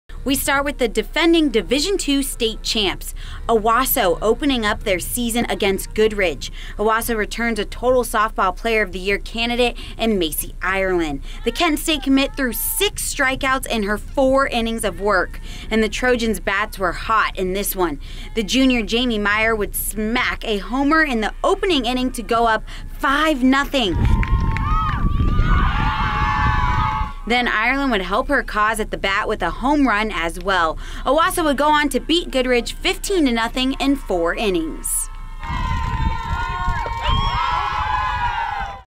Goodrich at Owosso - Softball | 4-11-22 | STATE CHAMPS! Michigan
🥎 Sydney Cariel  brings you the highlights of the Owosso vs. Goodrich softball game from April 11th, 2022